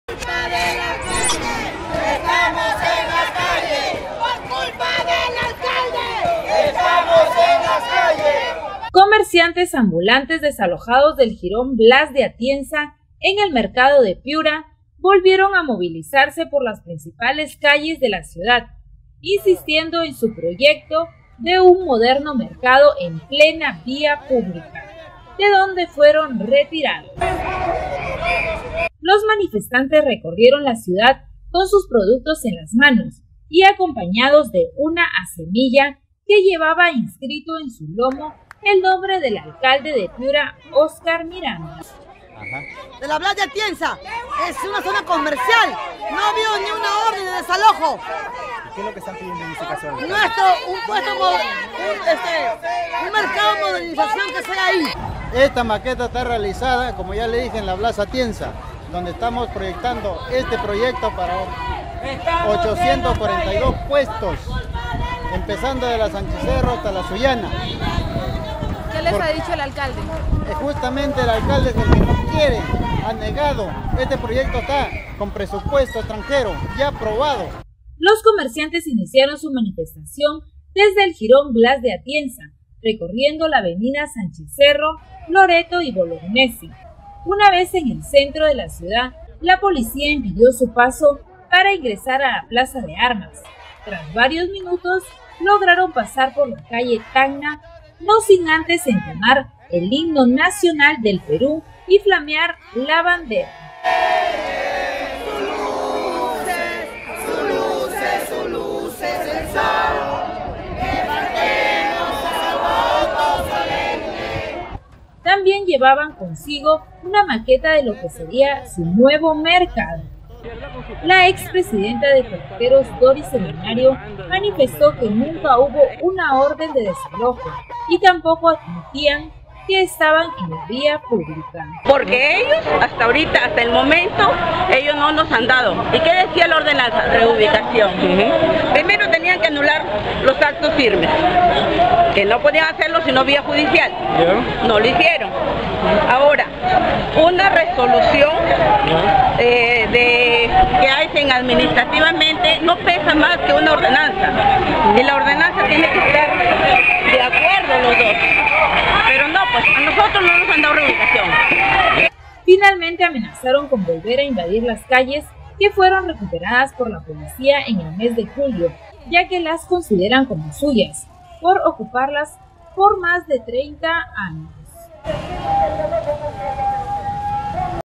COMERCIANTES DESALOJADOS SE MOVILIZAN
COMERCIANTES INSISTEN  EN CONSTRUIR UN MODERNO MERCADO EN PLENA VÍA  PÚBLICA.